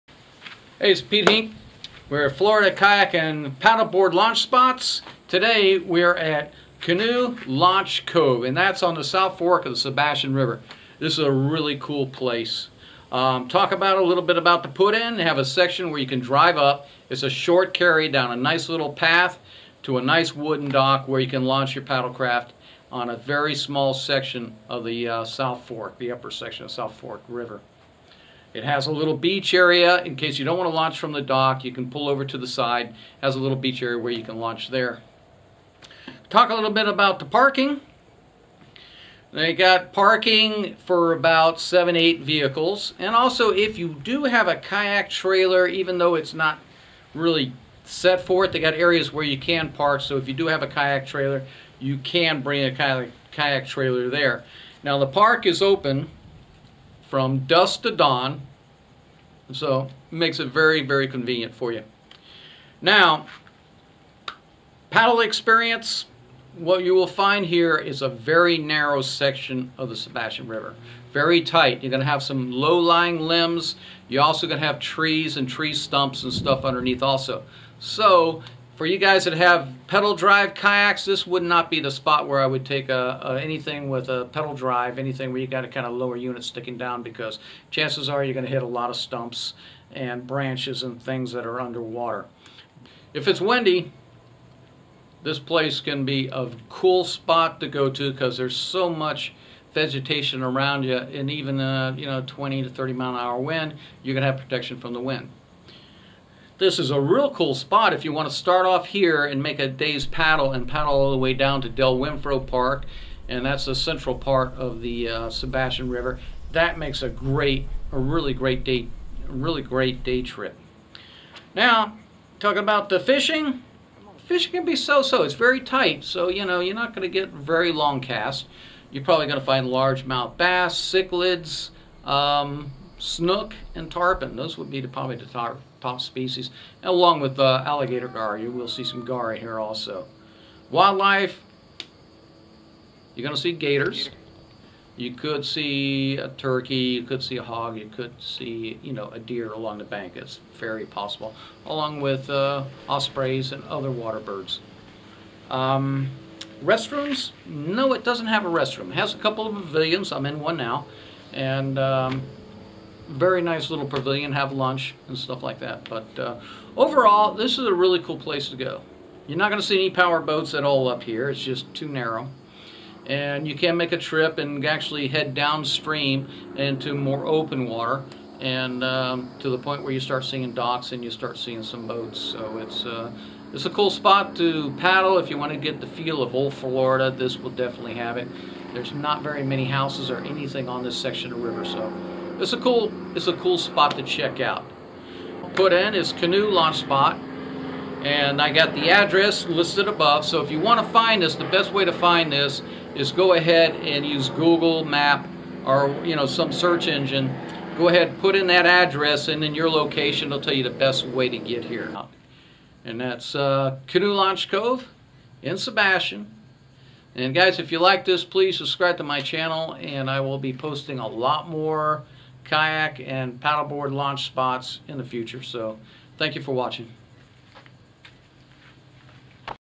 Canoe Launch Cove
Canoe Launch Cove is on the upper section of the Sebastian River. It is a great place to paddle and see Florida as it was 100 years ago.  Don't be supersized if you see a gator.